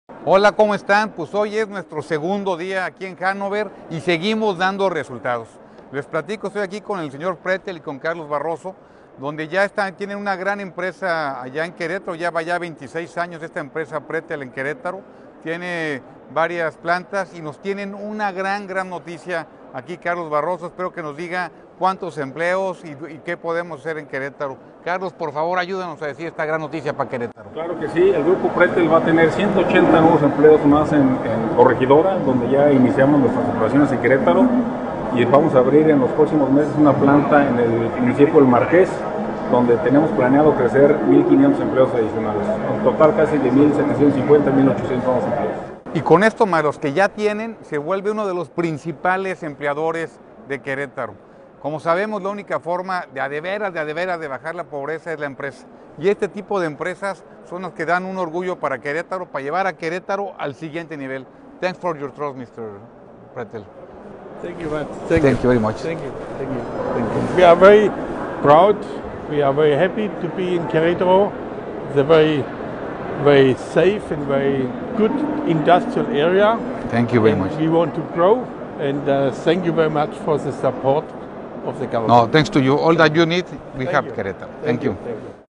Seguimos llevando buenas noticias para Querétaro : Mauricio Kuri Gobernador.
Como se los comentaba, seguimos llevando buenas noticias para Querétaro. Acabamos de cerrar una inversión más para nuestro estado con PRETTL Beteiligungs Holding, empresa que ya tiene 26 años en nuestro estado.

La inversión traerá 180 nuevos empleos en Corregidora y una nueva planta en El Marqués, para generar 1500 empleos para las y los queretanos. Vamos JuntosAdelante.